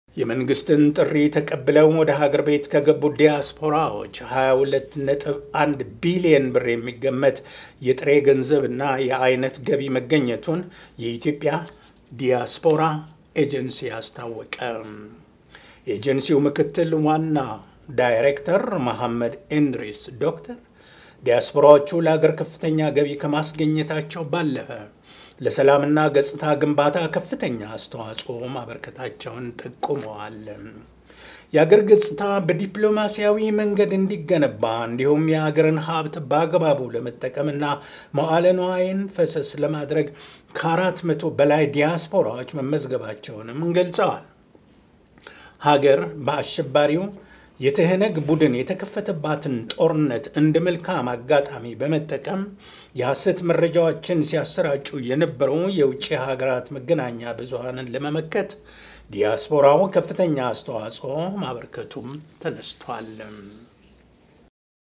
የመንግስት ጥሪ የተቀበሉት ኢትዮጵያውያን
የመንግሥትን ጥሪ ተቀብለው ወደ አገር ቤት ከገቡት ኢትዮጵያውያን 22 ነጥብ 1 ቢሊዮን ብር ሃብት ተገኘ።